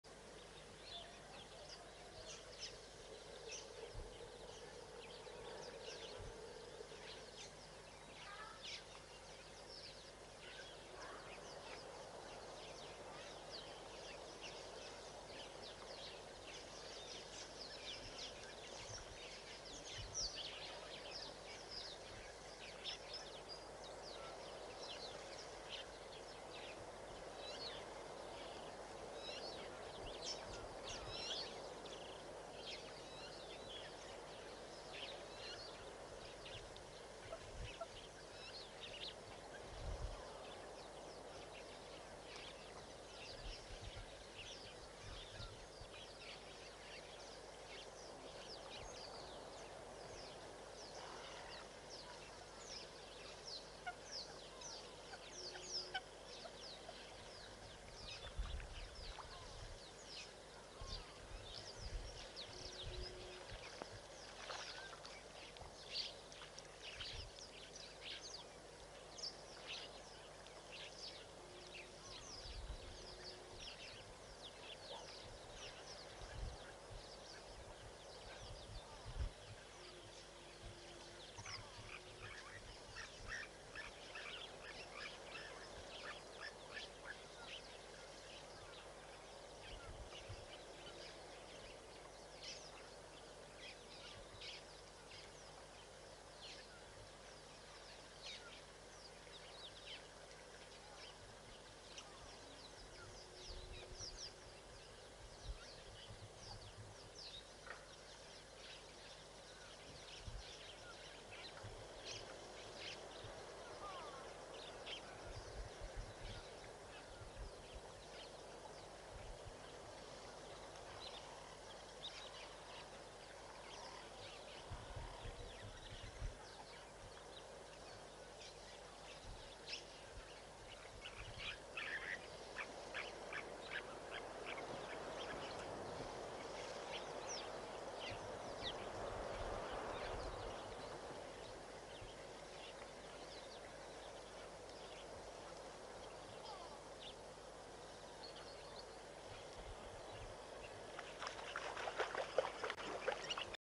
Lonely Flamingo. Explore.org 08 September 2021
The lonely juvie Flamingo is still at Tau Waterhole. :-)